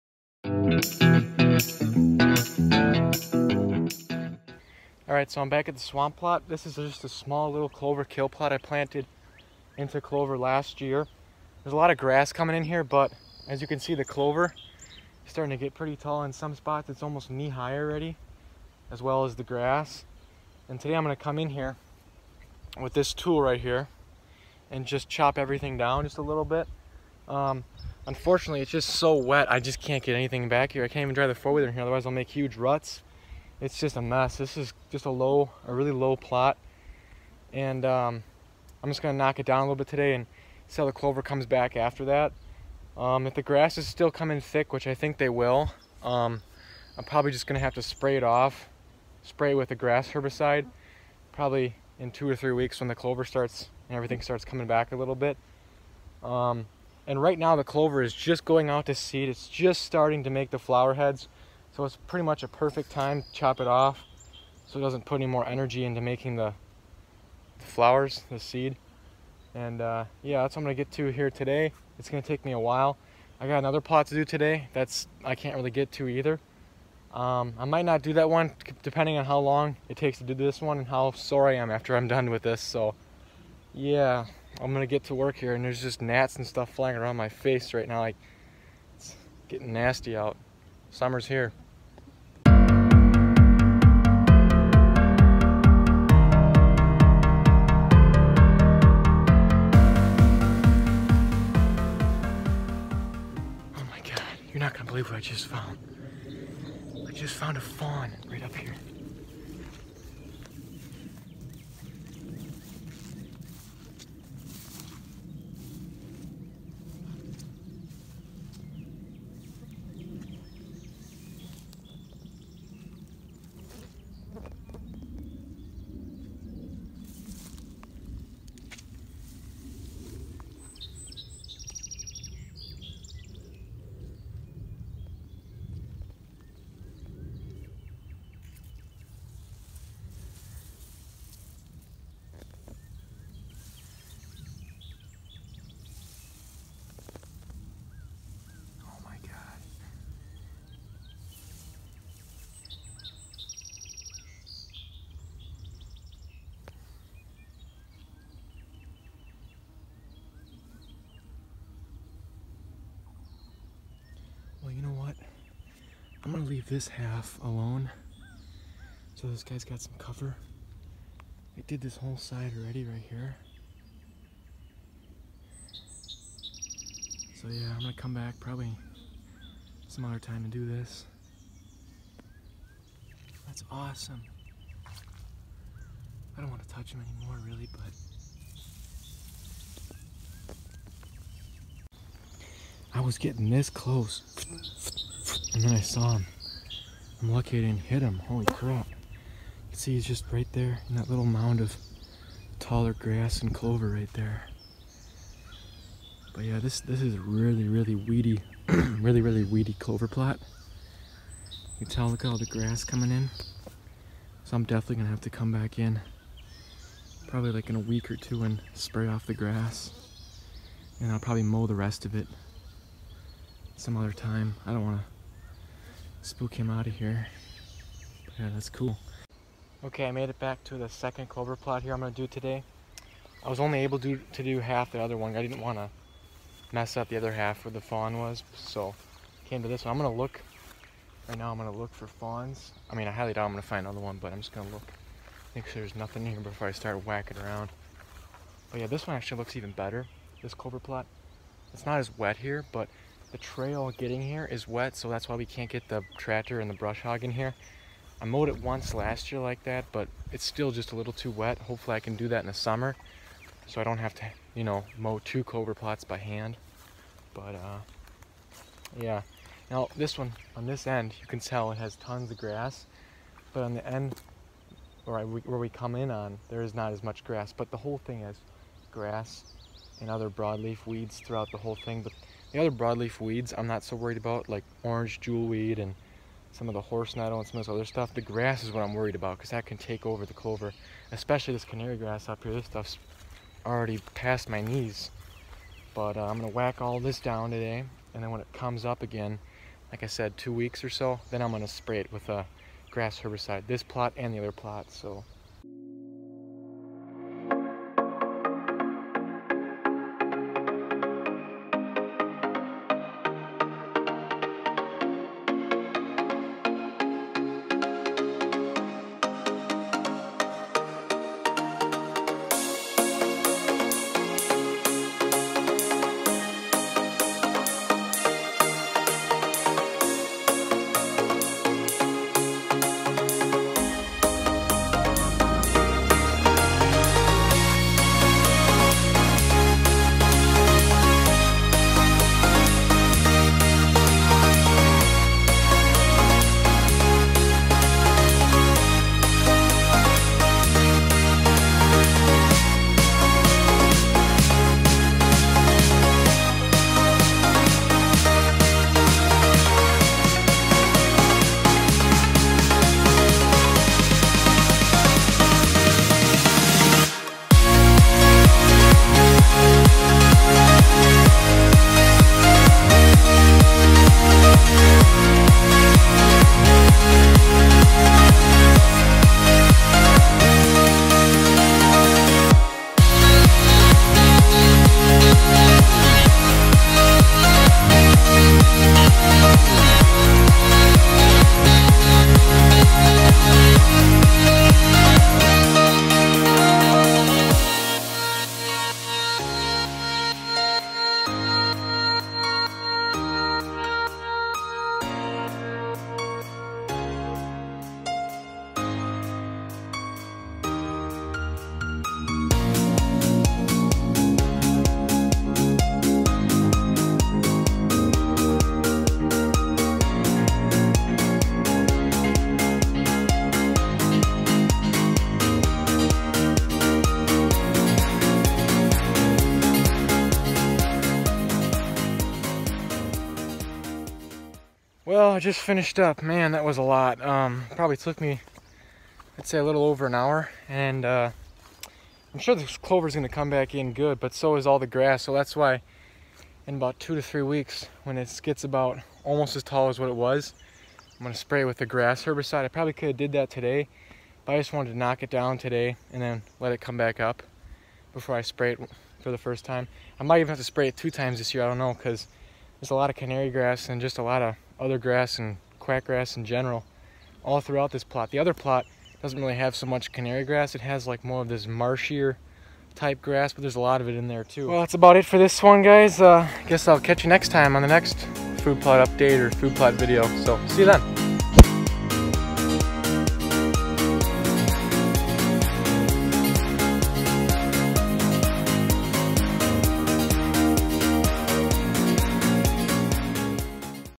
Mowing Clover Food Plots - Fawn in the Clover!
In this video I am mowing 2 clover plots by hand because it’s been so wet I can’t get the tractor and brush hog to them without ripping up the plots. Lots of grass is coming in unfortunately in both plots so I will need to spray them with a grass herbicide soon. That will be in an upcoming video in the next week or two. Clover plots can be great spots to hunt but they need to be maintained throughout the year by mowing and spraying out the weeds if you want them to last more than 2 years.